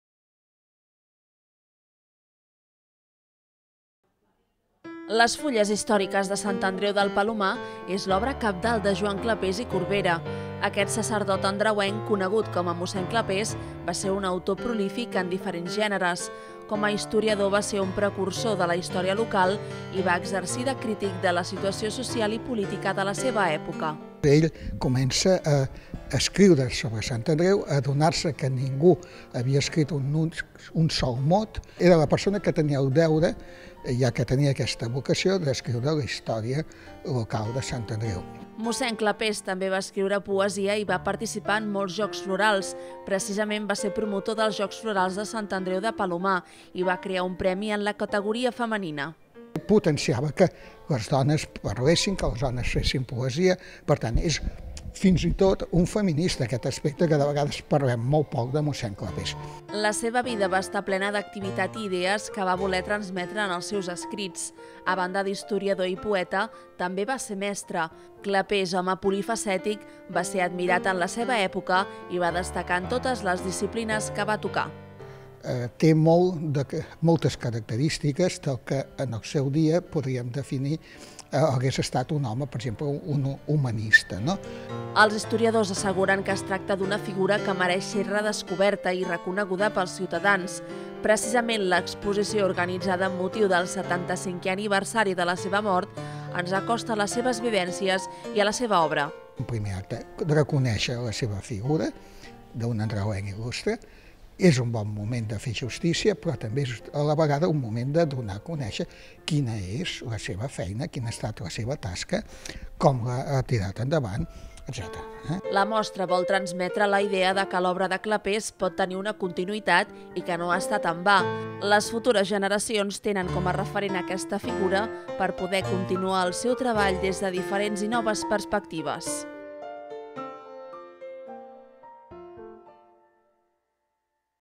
Joan Clapés i Corbera, il·lustre andreuenc
Aquest any es commemora el 75è aniversari de la mort de Joan Clapés i Corbera, sacerdot, escriptor, poeta i historiador. La figura i obra d'aquest religiós és cabdal per a les fonts historiogràfiques de Sant Andreu del Palomar. Més informació